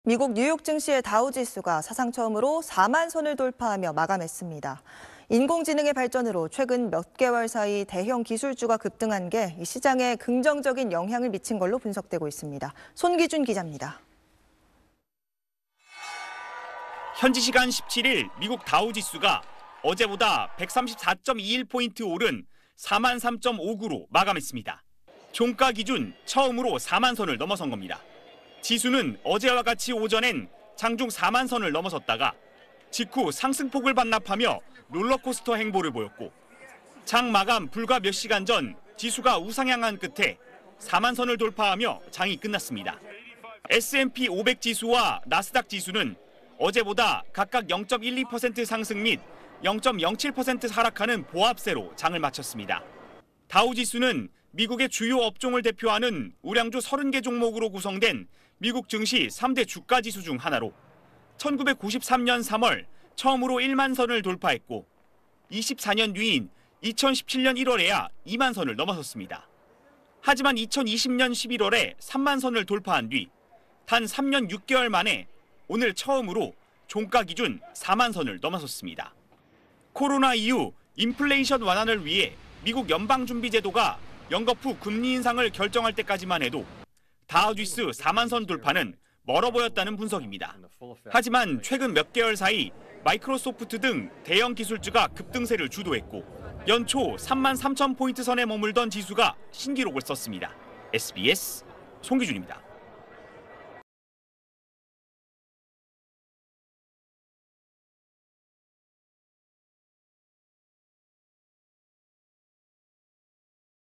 미 다우지수, 사상 첫 40,000선 돌파 마감 / SBS
〈앵커〉

미국 뉴욕증시의 다우 지수가 사상 처음으로 4만 선을 돌파하며 마감했습니다. 인공 지능의 발전으로 최근 몇 개월 사이 대형 기술주가 급등한 게 시장에 긍정적인 영향을 미친 걸로 분석되고 있습니다.

손기준 기자입니다.

〈기자〉

현지시간 17일 미국 다우지수가 어제(17일)보다 134.21포인트 오른 40,003.59로 마감했습니다.

종가 기준 처음으로 4만 선을 넘어선 겁니다.

지수는 어제와 같이 오전엔 장중 4만 선을 넘어섰다가 직후 상승 폭을 반납하며 롤러코스터 행보를 보였고, 장 마감 불과 몇 시간 전 지수가 우상향한 끝에 4만 선을 돌파하며 장이 끝났습니다.

S&P500지수와 나스닥 지수는 어제보다 각각 0.12% 상승 및 0.07% 하락하는 보합세로 장을 마쳤습니다.

다우지수는 미국의 주요 업종을 대표하는 우량주 30개 종목으로 구성된 미국 증시 3대 주가지수 중 하나로, 1993년 3월 처음으로 1만 선을 돌파했고, 24년 뒤인 2017년 1월에야 2만 선을 넘어섰습니다.

하지만, 2020년 11월에 3만 선을 돌파한 뒤, 단 3년 6개월 만에 오늘 처음으로 종가 기준 4만 선을 넘어섰습니다.

코로나 이후 인플레이션 완화를 위해 미국 연방준비제도가 연거푸 금리 인상을 결정할 때까지만 해도, 다우지수 4만 선 돌파는 멀어 보였다는 분석입니다.

하지만, 최근 몇 개월 사이 마이크로소프트 등 대형 기술주가 급등세를 주도했고, 연초 3만 3천 포인트 선에 머물던 지수가 신기록을 썼습니다.

(영상편집 : 우기정)